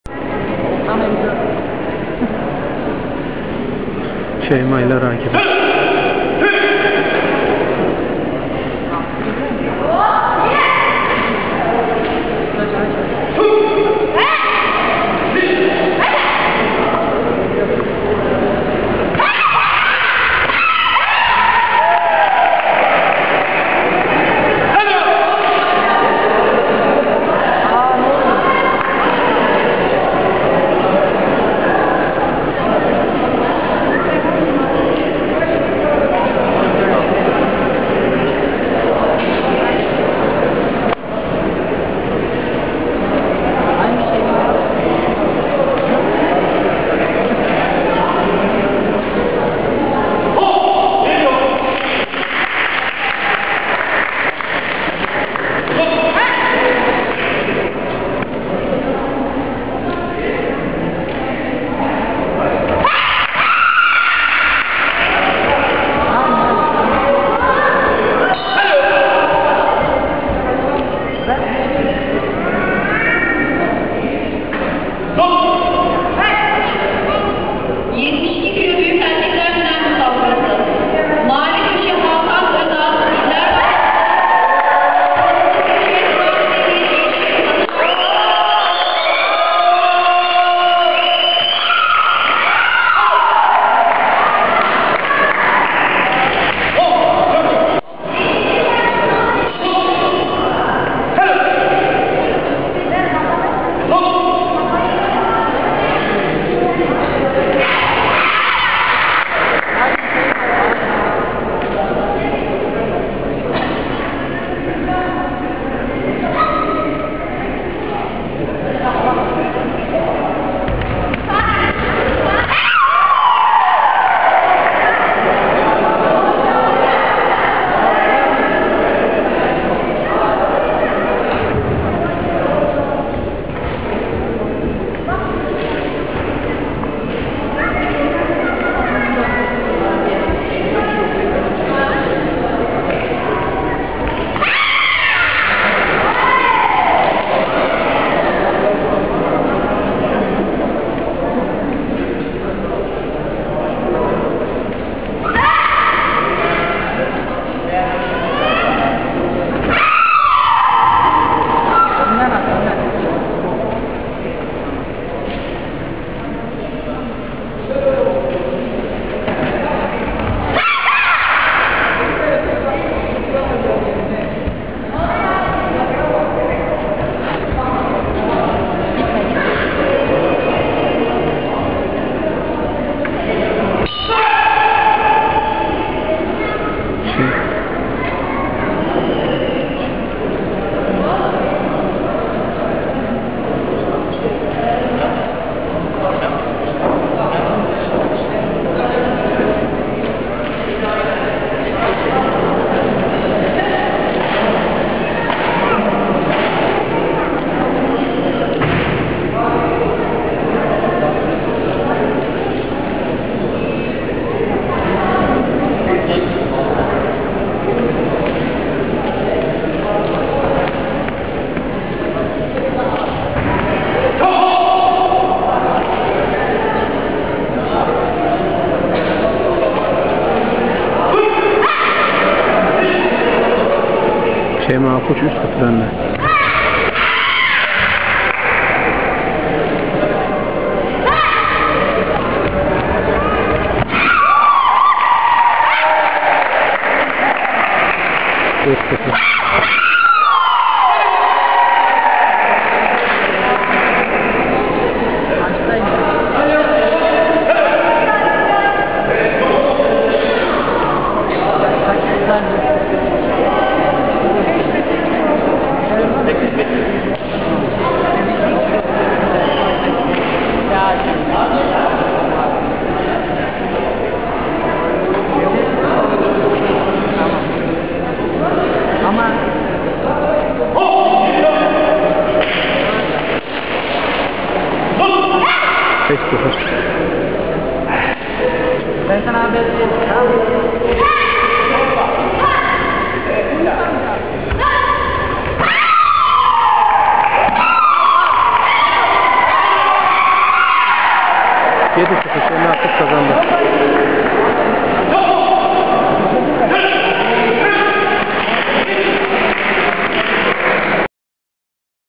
Ankara 30 Agustos Kupası 63KG Bayanlar Final
63KG
1-Şeyma Akkuş    İLLER BANKASI
2- Dilek Gülnaz    ÇAĞLAR